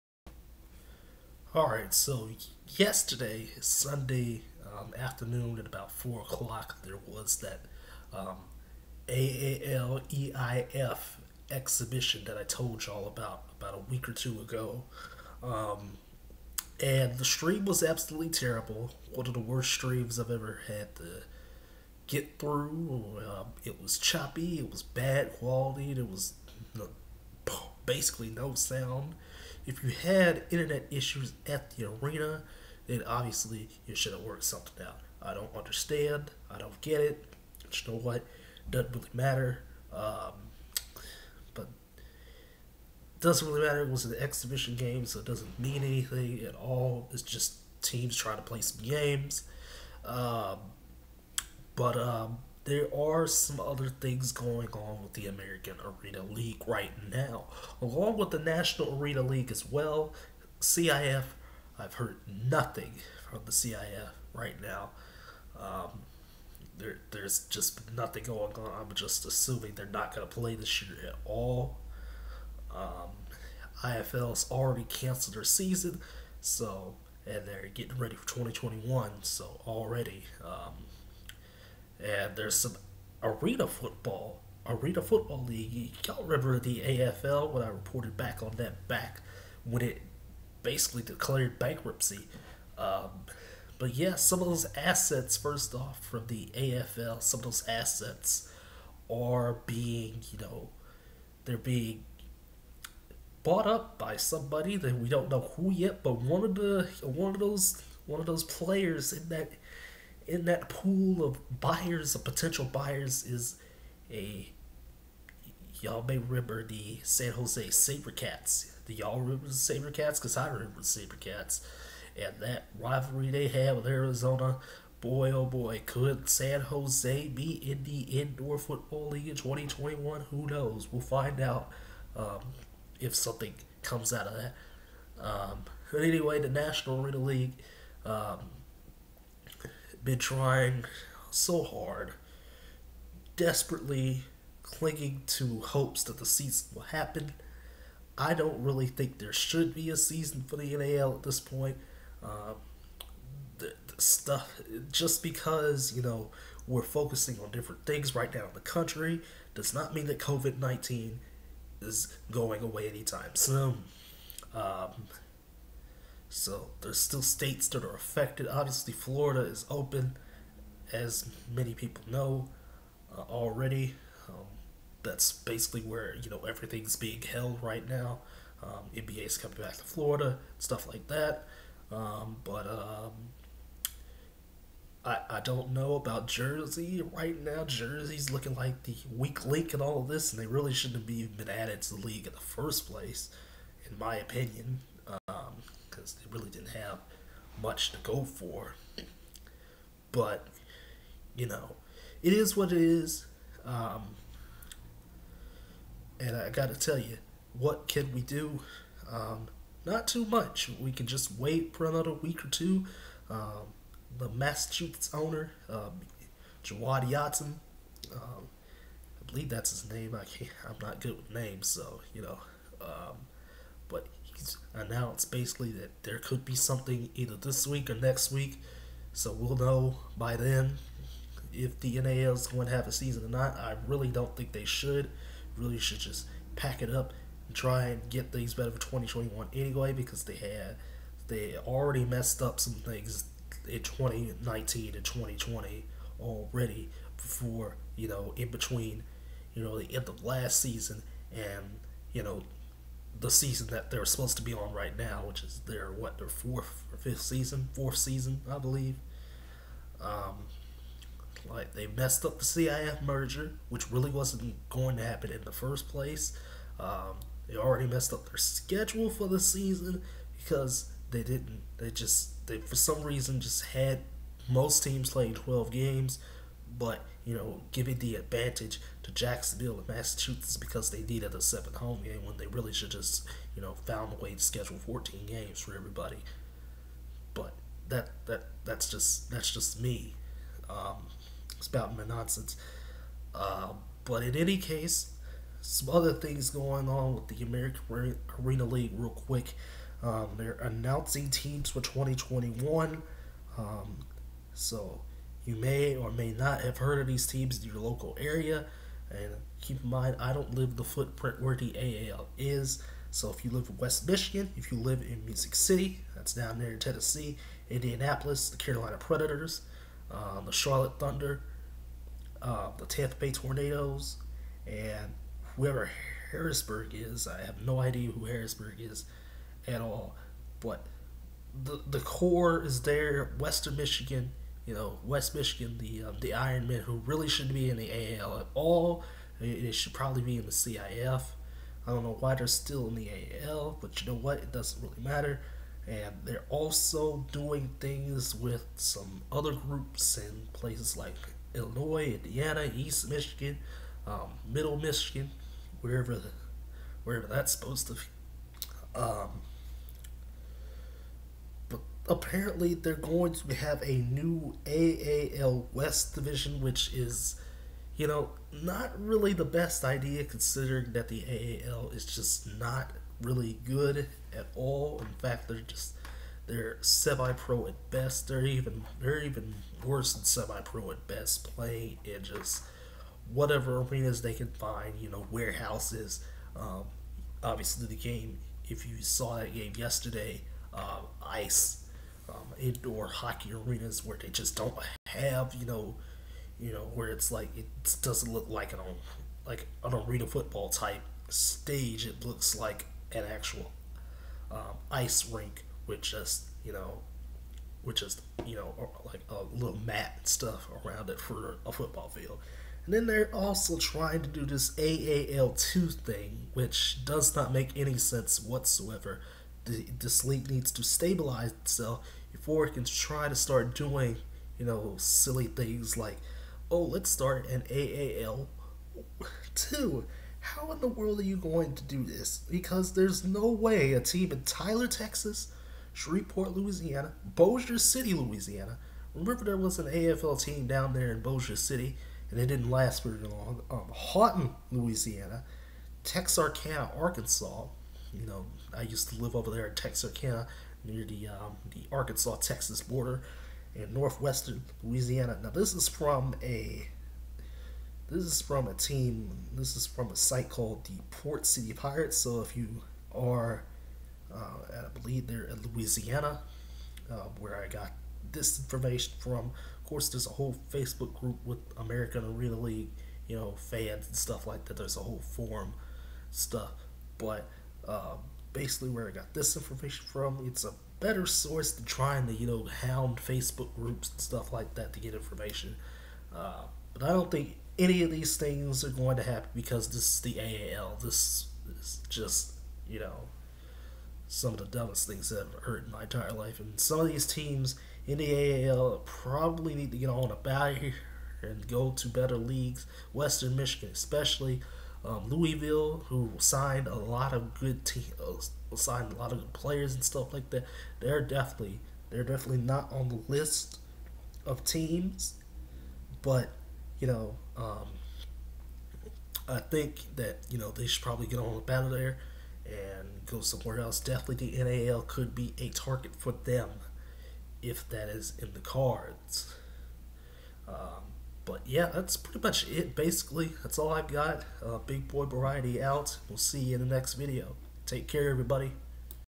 Indoor/Arena Football Landscape June 2020
0:00 Southern Steam/Carolina Exhibition
1:20 AFL Assets...San Jose Sabercats Return?
2:15 National Arena League Delays Season Start Again
4:55 American Arena League Delusional Again